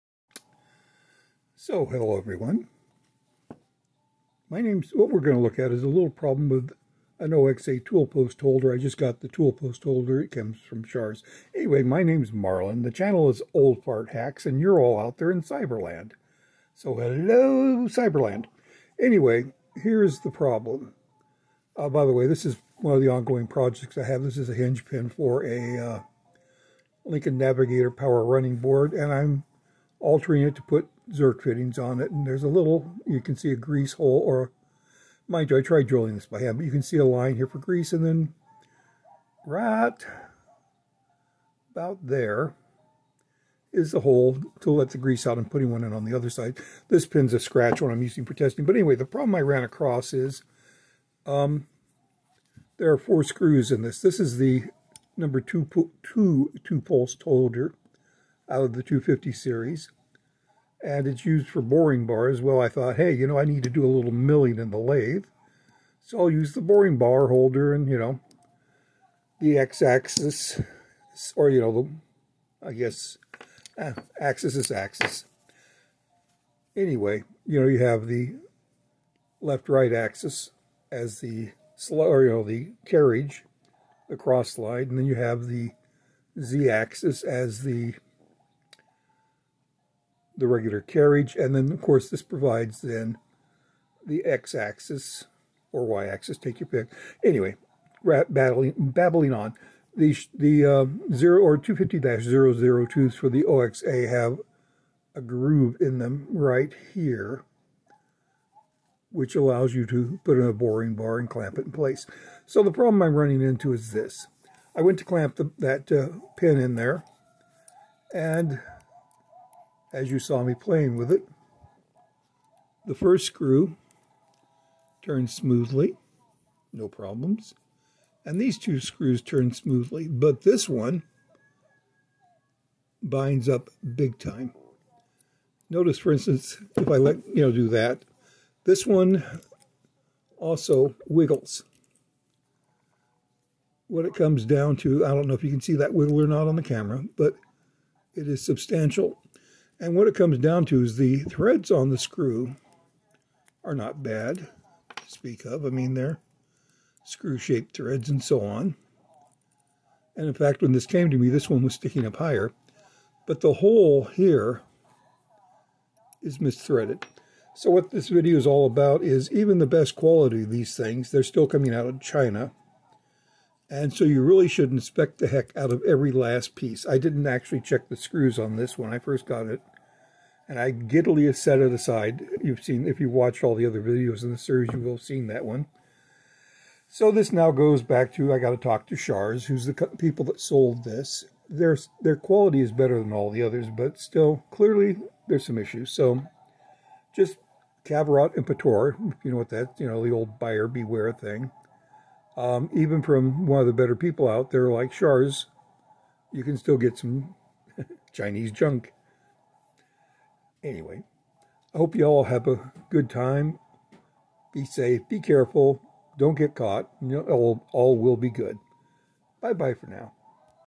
Shars OXA 250 002 tool holder problem, bad thread on one of the 4 clamp screw holes.
So I needed to use one of the other holders for my Shars OXA quick change tool post, the 250-002 holder that takes regular and round shank tools.

In this video, I go into what I  was doing, that being setting up to drill a round mount pin through the side of the pin. As I was doing that, I found a problem with the holder. This was my first time using this holder.

There are 4 clamping screws on this holder, the 2nd from the working end has a bad thread in the holder.

So the upshot is, when you get a new tooling system with this many pieces, check them all.

I will be getting in contact with Shars today to see how they want to handle the problem. I will update on that as things go on.